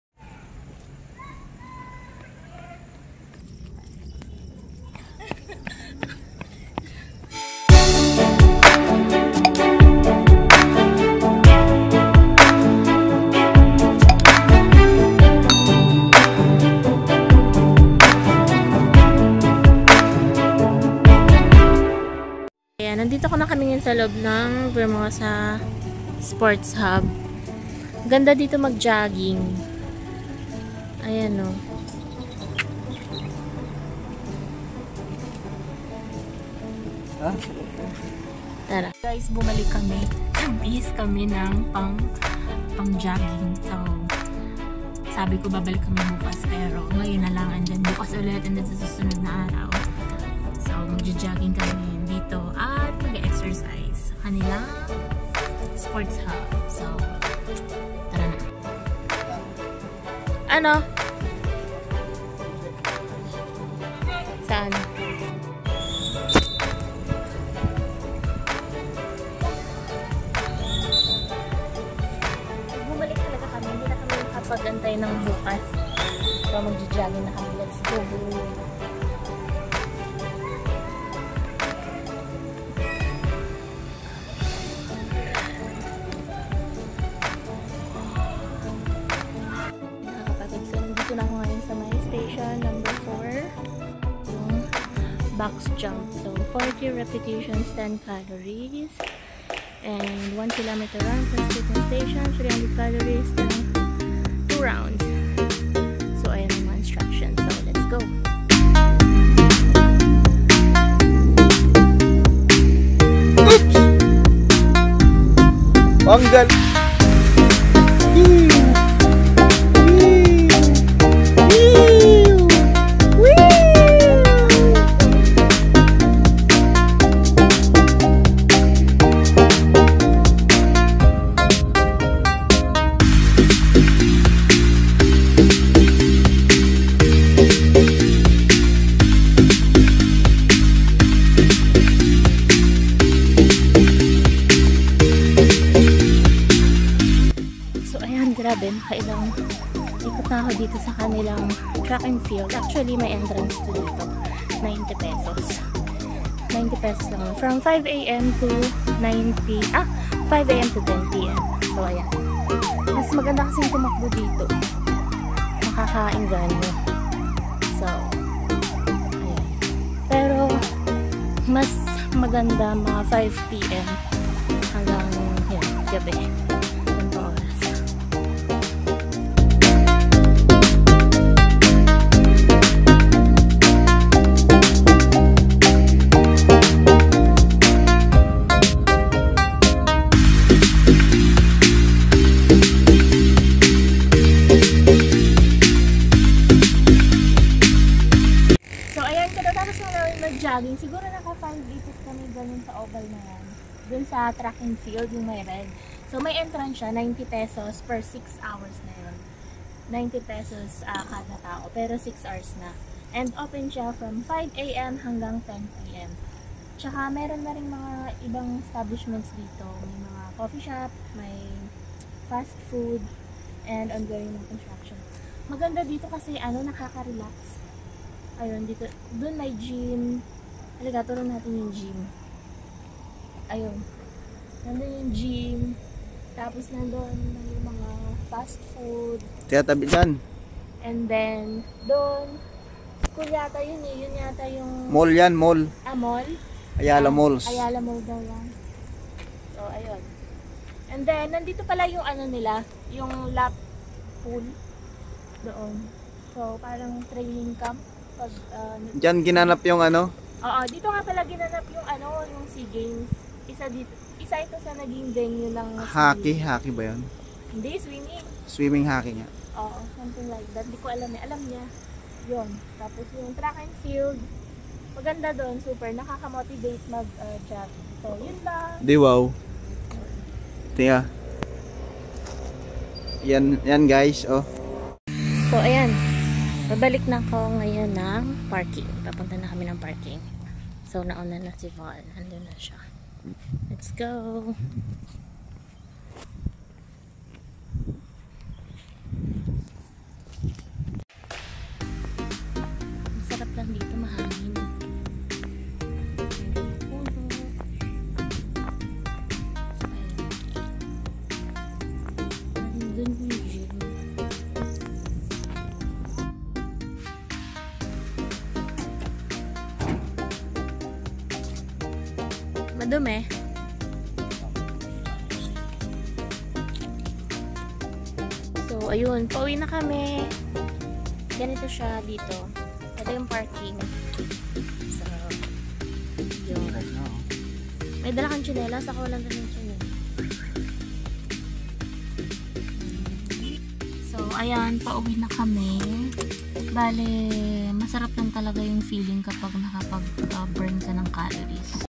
VERMOSA SPORTS HUB | Bagong Tambayan sa DAANGHARI
Vermosa Sports Hub Vlog. Hi guys! welcome back to the vlog. Nagpunta kami ng Vermosa at we had so much fun exercising. We're able to run on an international standard track oval. Meron din silang olympic size lap pool, fitness laboratory, football field and maaliwalas na paligid. One of the best along Daanghari.